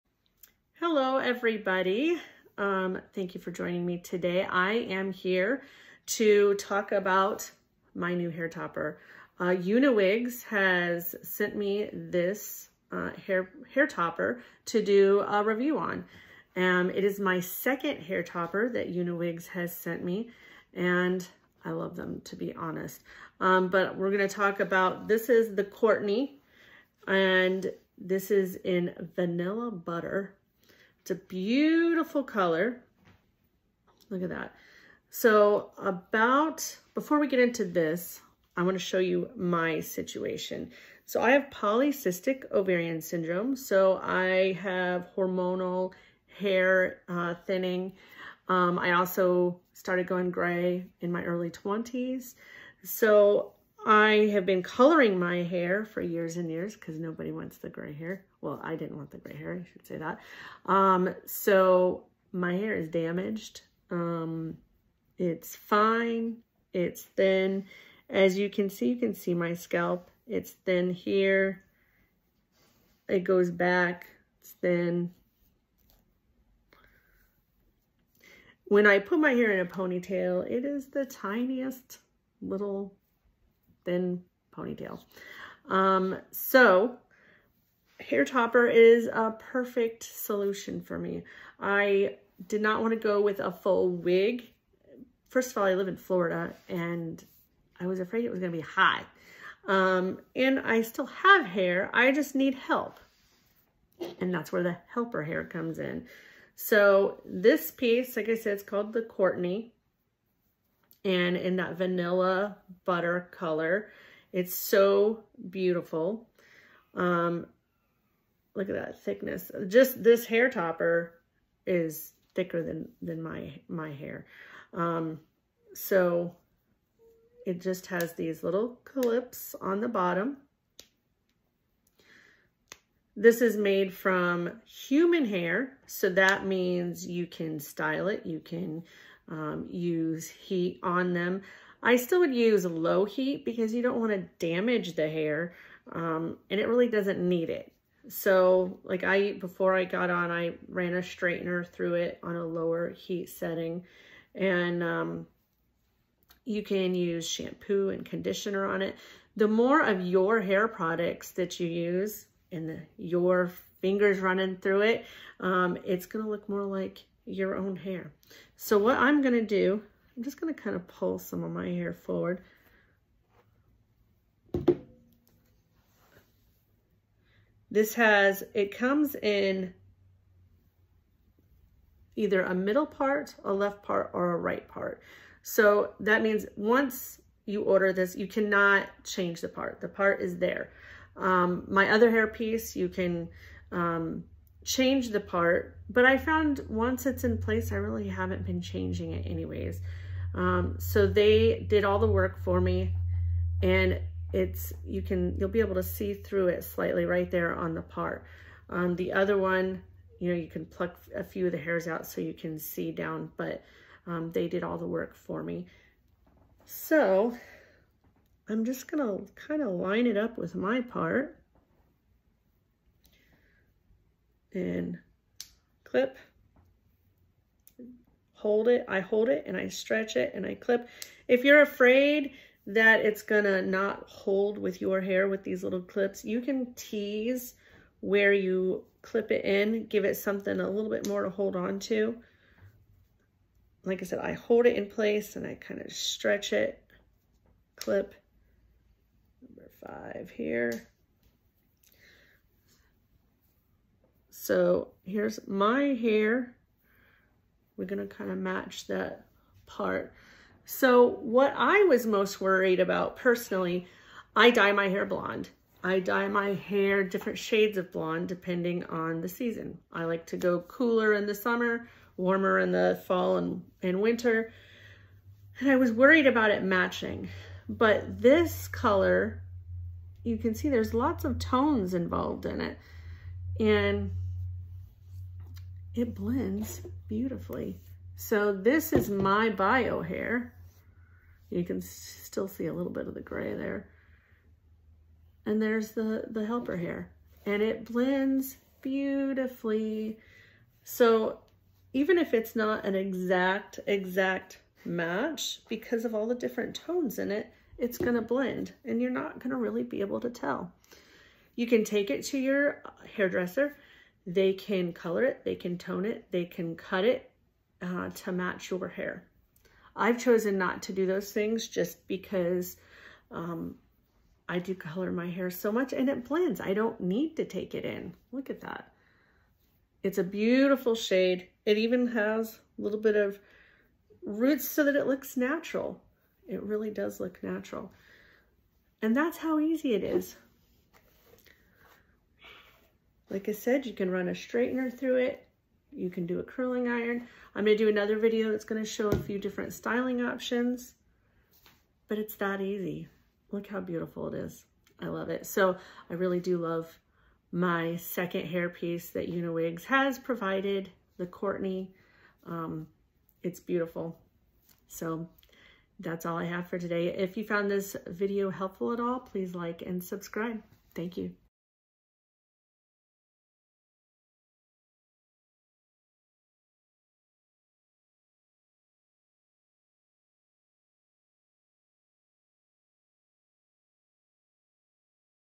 My new “Courtney” helper hair from UniWigs.
My review of my new helper hair.

7"X8" COURTNEY | MONO PART | LEFT PART REMY HUMAN HAIR TOPPER:
Color: 626R
UniWigs hair topper collection Instagram: @uniwigs.helperhair